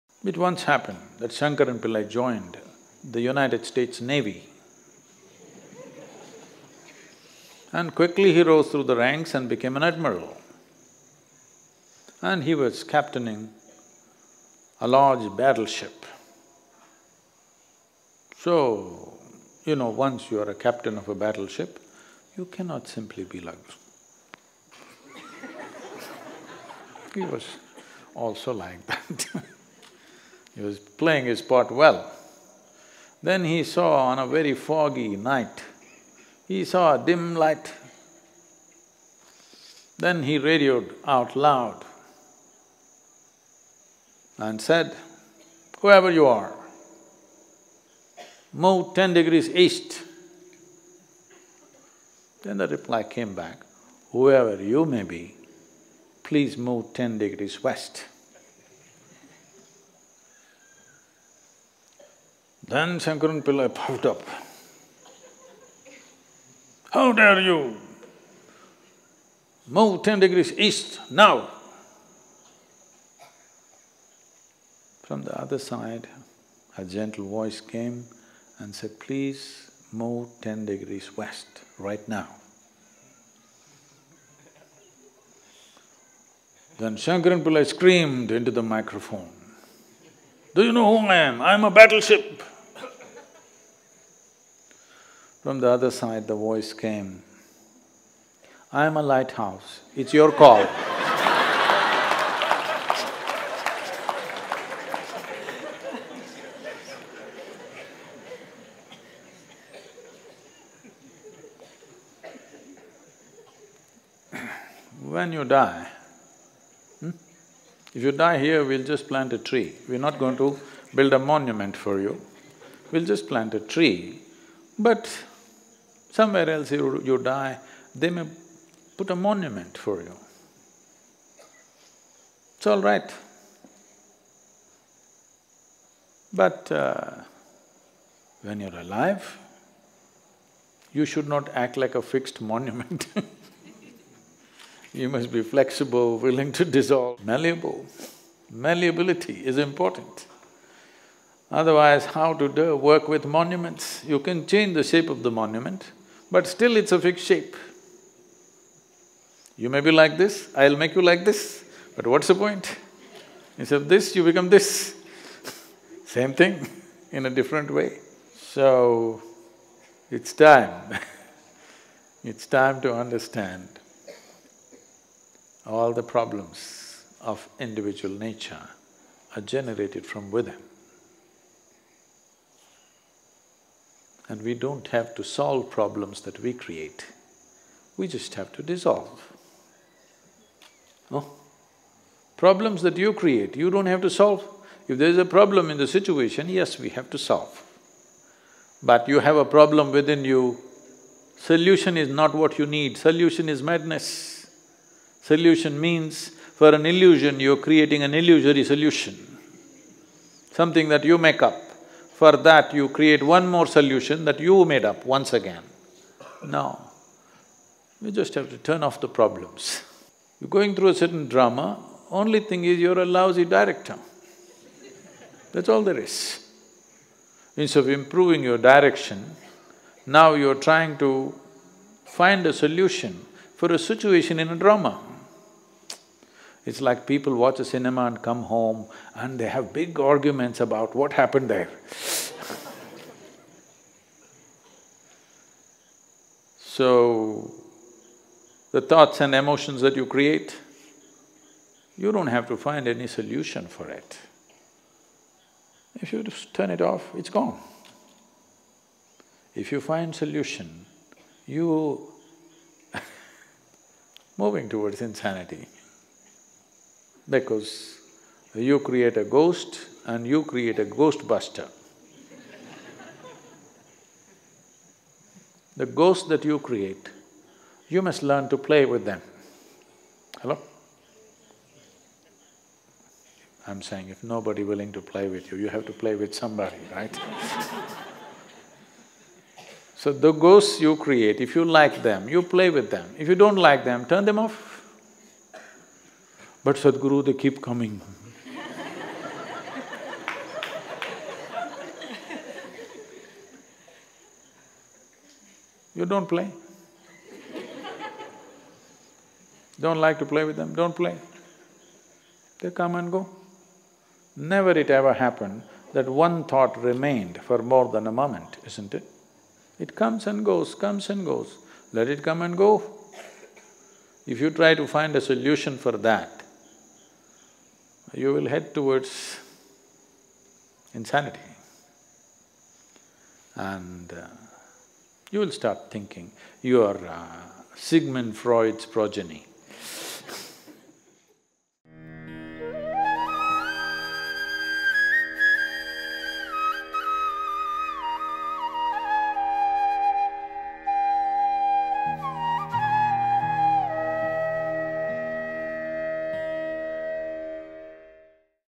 How to Dissolve the Drama of Life? | Sadhguru
If your life has become a bad drama, says Sadhguru, it just means you’re a lousy director. The problems of individual nature, he explains, come from within. Instead of creating an illusory solution for your own illusion, it’s time to switch off the problems altogether.